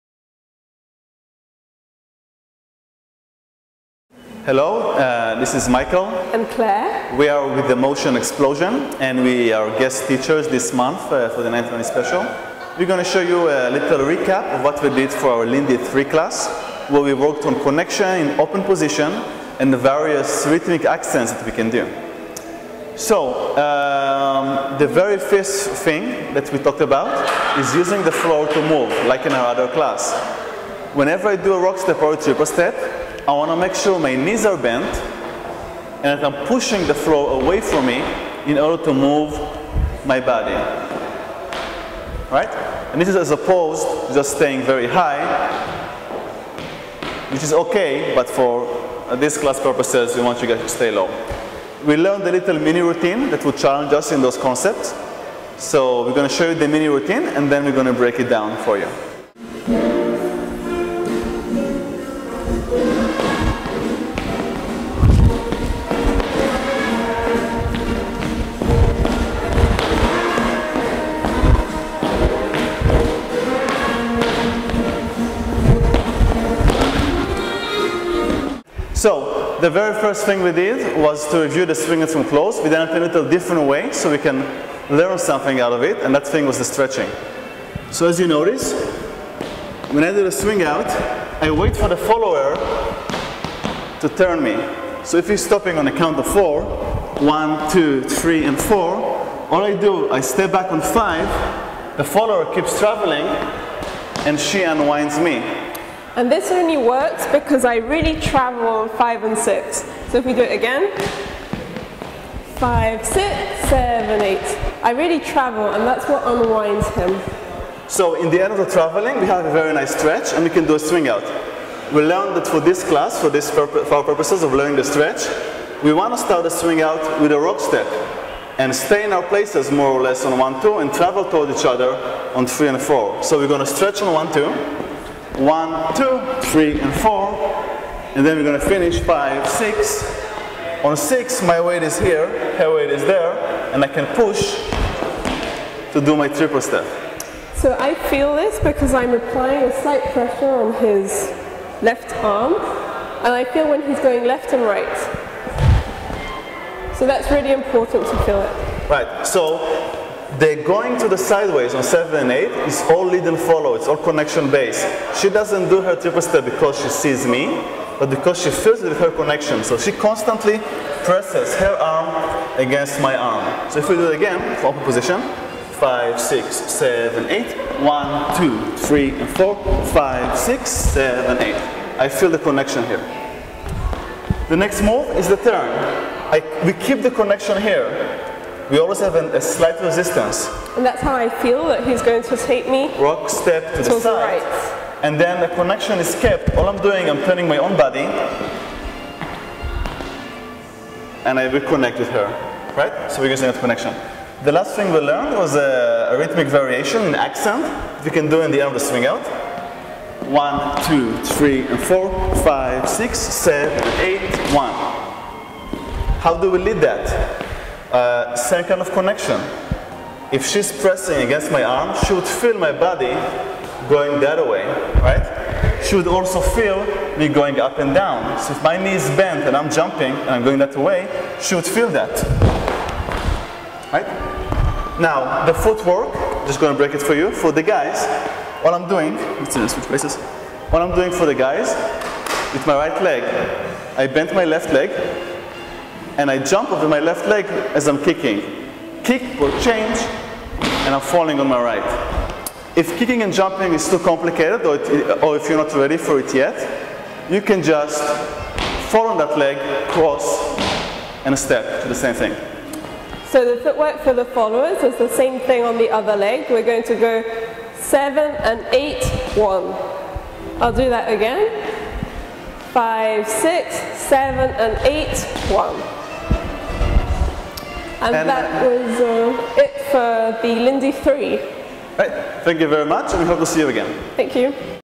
April 2011 — Level 3 Lindy — Week 2
Instructors: Michael Terkowski and Claire Oldfield from The Motion Explosion Dance Troupe

Michael and Claire review the footwork and variations from this week's Level 3 class: Fancy Moves!